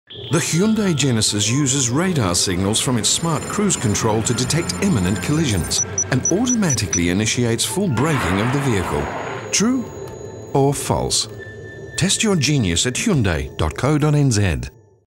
Hyundai Genesis - Autonomous Emergency Braking
Hyundai Genesis - Inbuilt Genius

The Hyundai Genesis uses radar signals from its smart cruise control to detect imminent collisions, and automatically initiates full braking of the vehicle.

True? Or False?

Competition ends 31st March, 2015